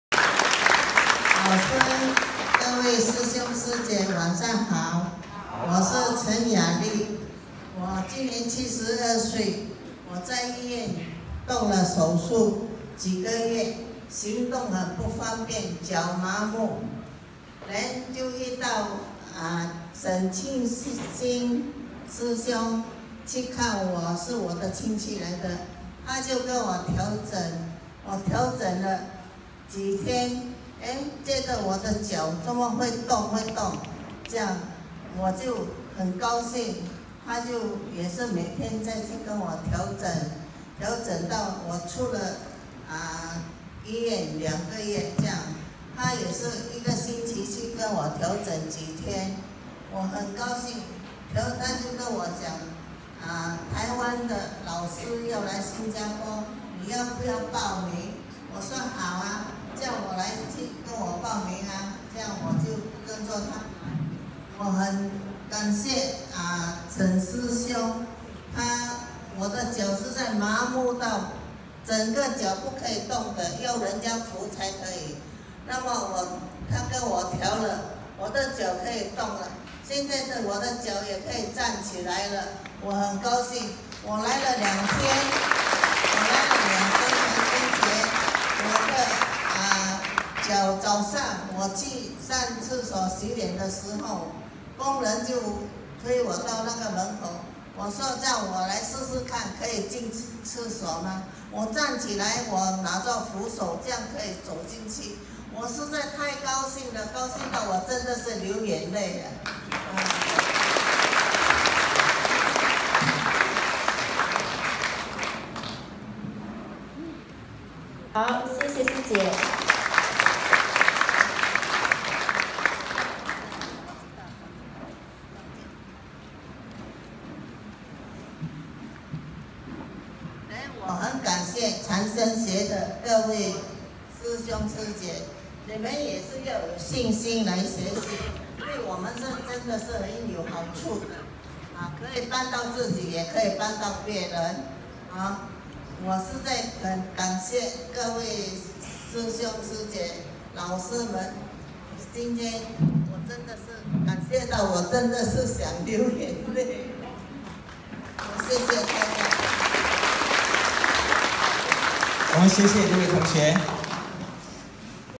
開刀後遺症.腳麻到不能站起來走路（長生學見證系列）Longevitology
陈亞莉 同學見證分享：脊椎问題開刀完雙腳麻到不能动弹、經過調整几次后腳可以動了、能起身了。真是感恩長生學！2017年3月15日 新加坡